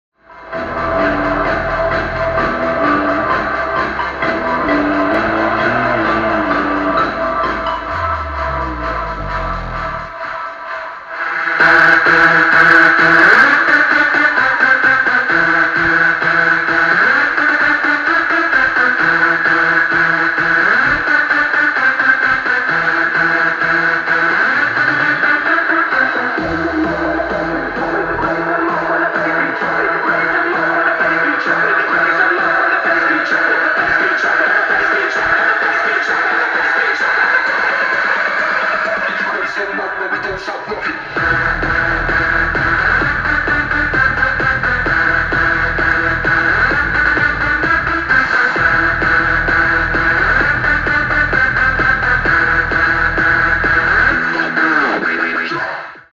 DJ Soundspace - AWFalia 2017
Oprawa muzyczna Juwenalii 2017 dla AWF-u.